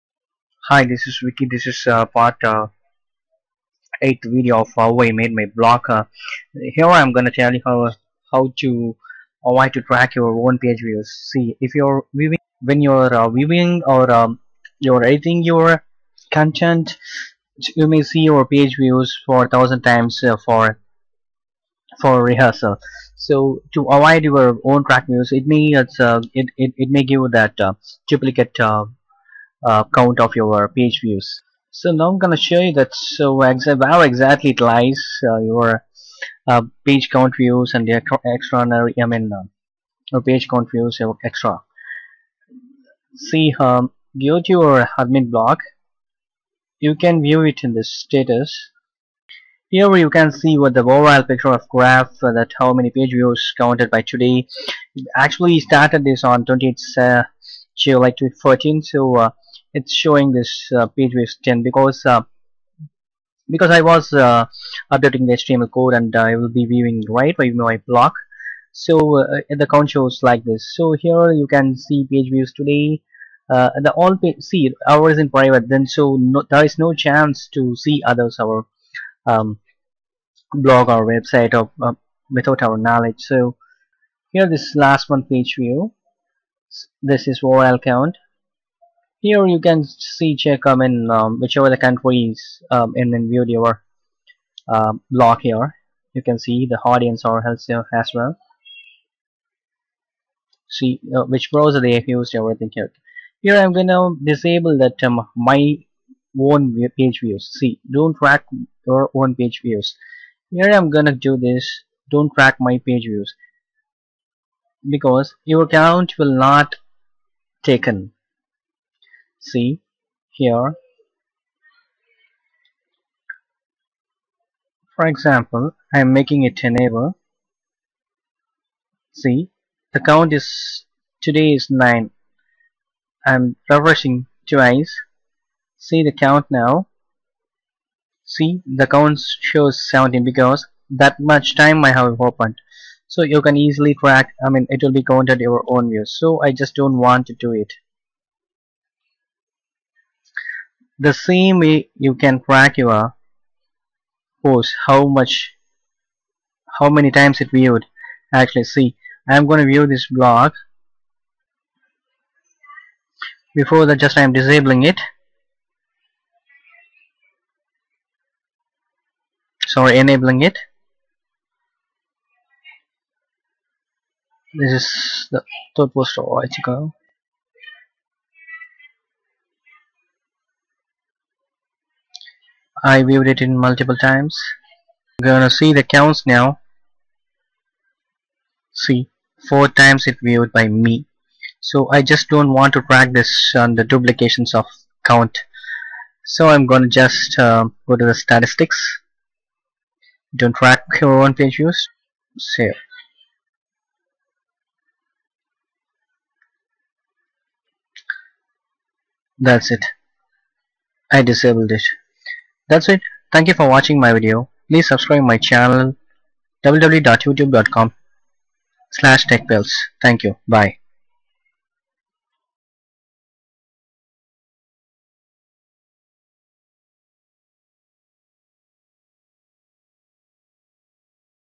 How I made my blog - Avoid to track your own page views - Part 8
This video is continuation of How I made my blog - How I made my blog - How to Add or Remove the Menu and Sub menus from html code-  Part 7

In this Part 8, I have explained about to track your page views of your blog or website and to avoid your own page view to remove the count duplication.

How I made my blog - How to Schedule your article to publish automatically? - Part 9